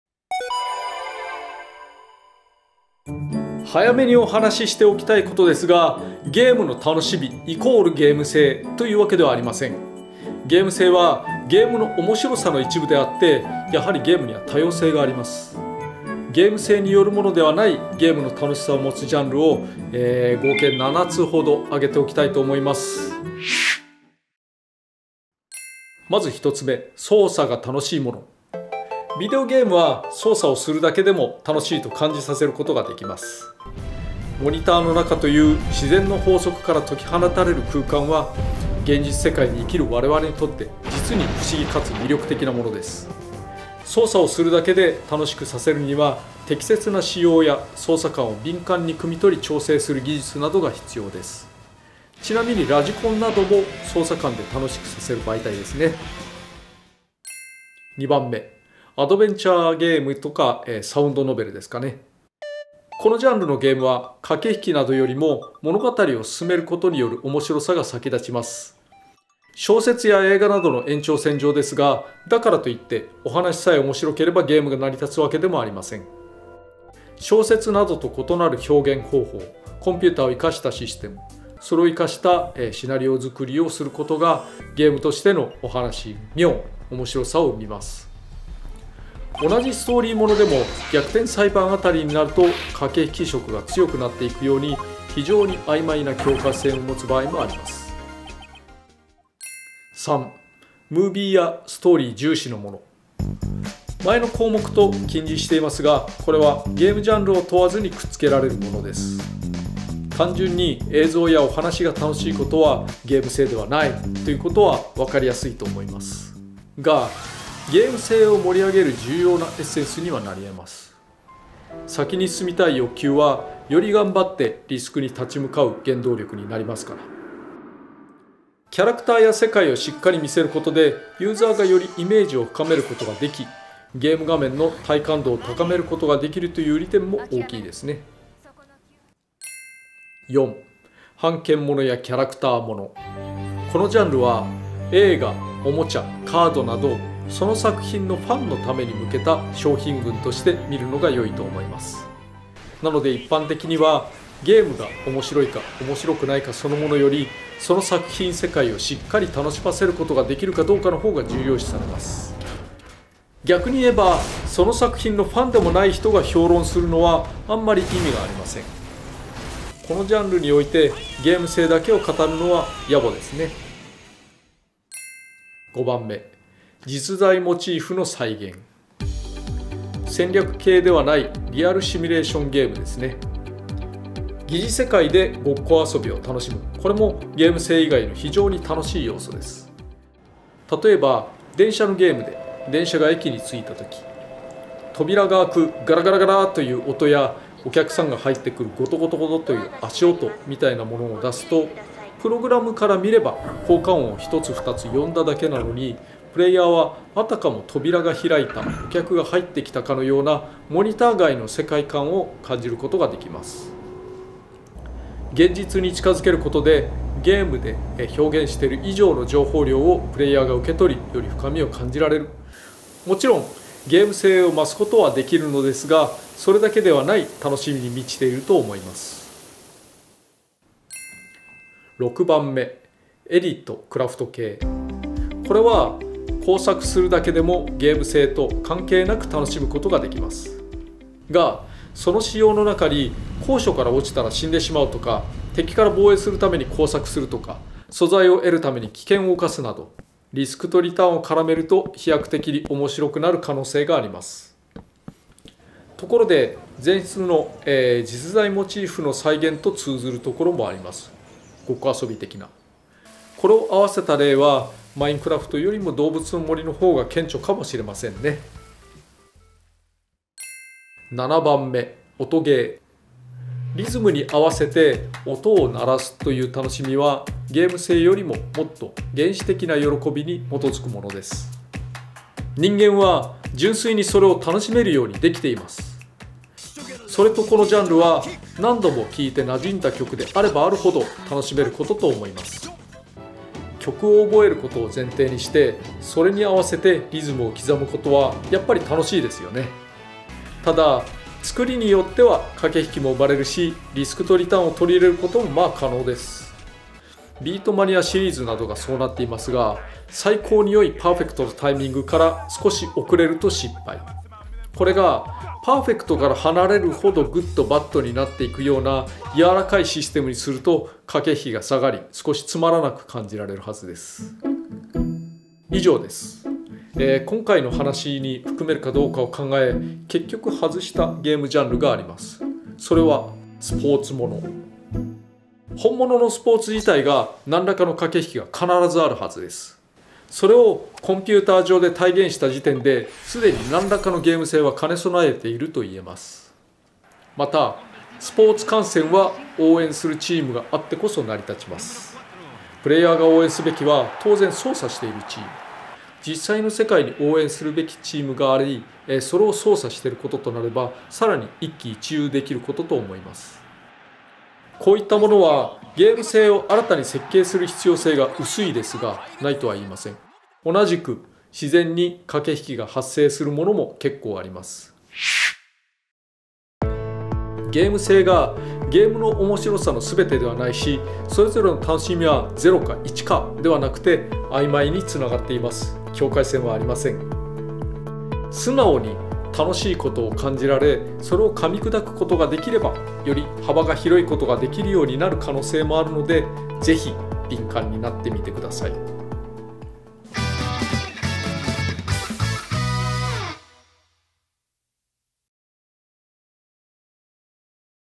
Fun Beyond Game Essence [Game Essence]
We've talked about "game essence" before, but game essence isn't the only thing that makes games fun!

In this video, I'll talk about some types of games that don't rely on game essence for fun.

The possibilities for fun in games are endless.

Sorry for the wait:

From this episode onward, the jingle at the start will be slightly quieter, and the titles of other developers' games will be displayed on-screen.

I took so much footage for this episode...